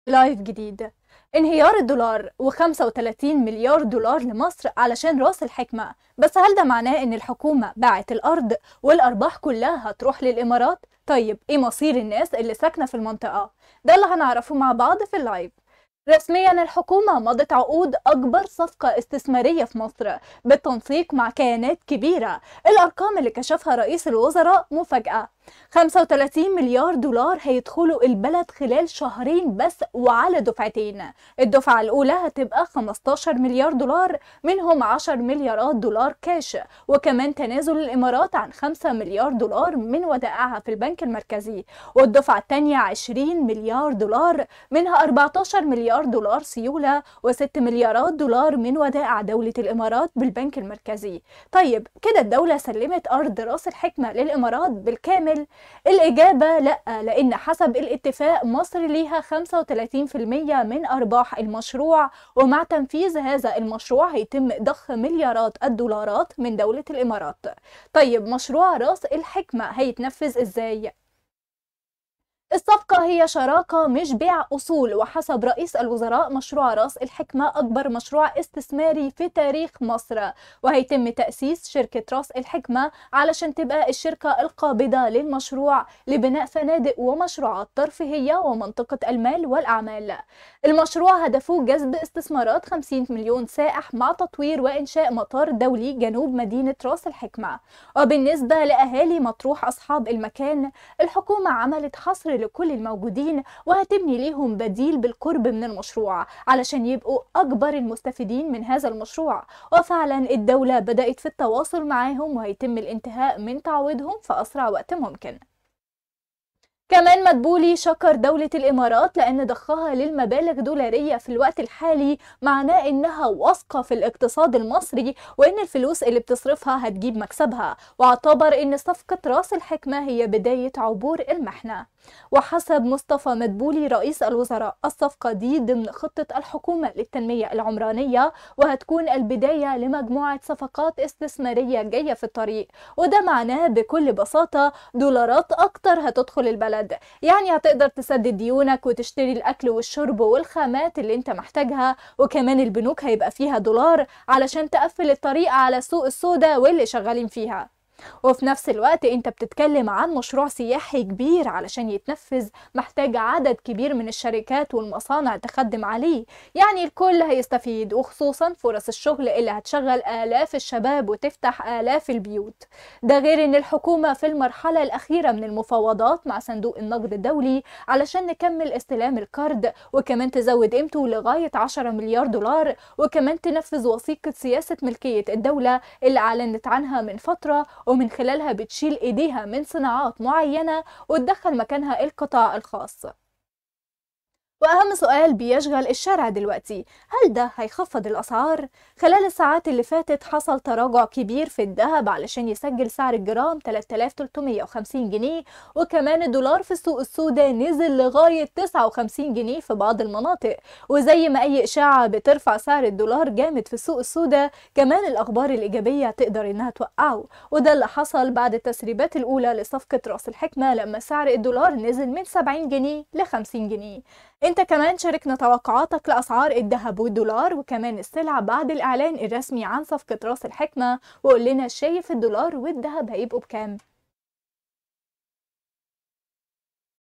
35 مليار دولار.. والدهب ينهار! تفاصيل صفقة رأس الحكمة.. فهل باعت الحكومة الأرض عشان الدولار؟
الصفقة هى شراكة مش  بيع أصول، وحسب رئيس الوزرا  مشروع رأس الحكمة أكبر مشروع استثماري في تاريخ مصر،  وهيتم تأسيس شركة رأس الحكمة  عشان تبقى الشركة القابضة للمشروع، لبناء فنادق ومشروعات ترفيهية، ومنطقة المال والأعمال،